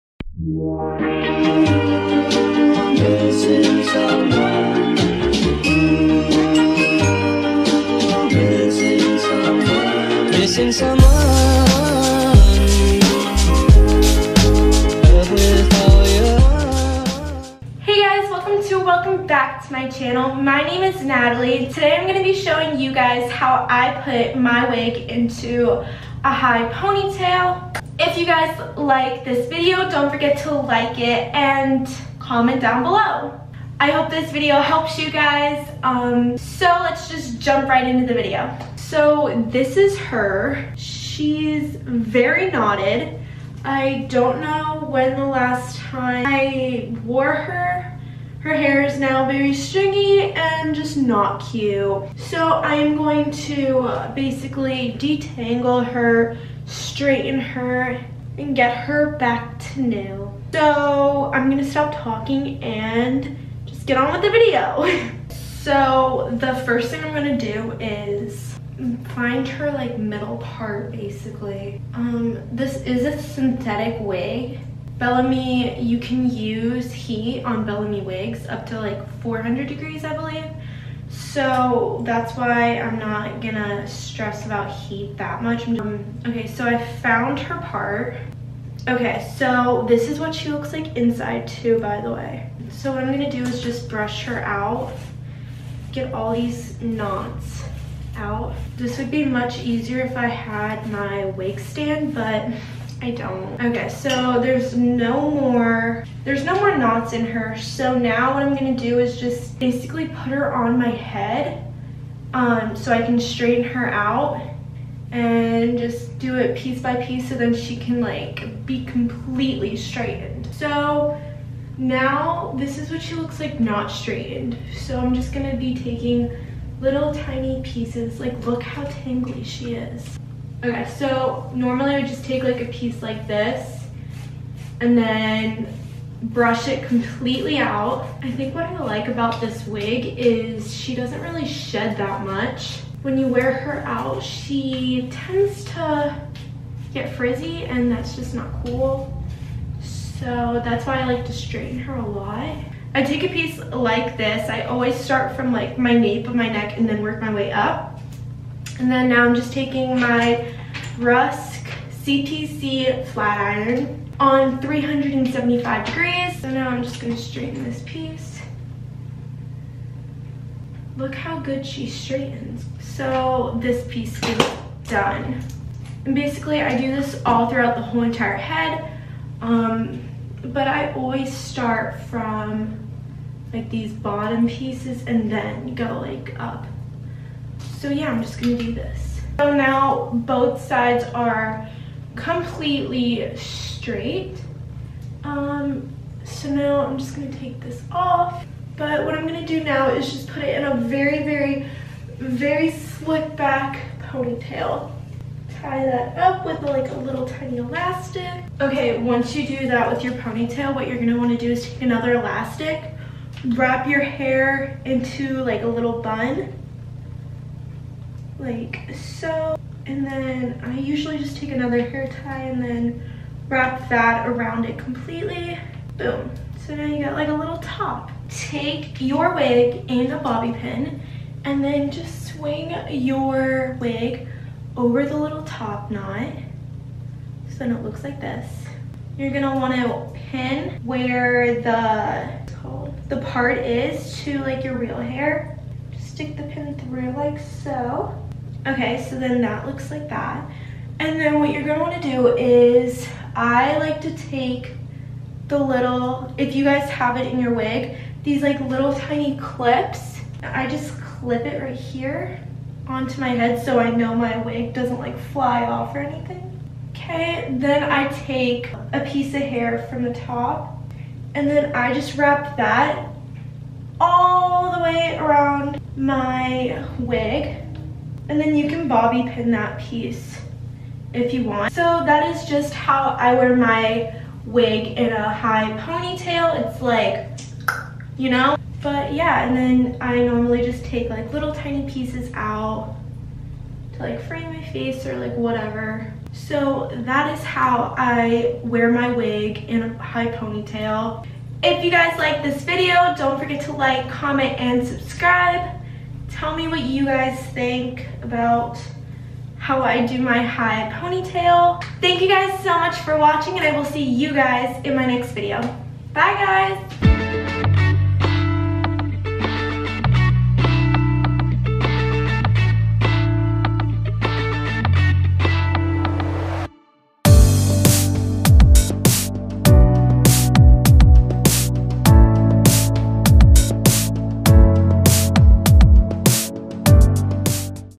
How to Put a Synthetic Wig into a High Ponytail
This is how to put a synthetic wig into a High Ponytail. I used my Bellami wig for this look! let me know what you think.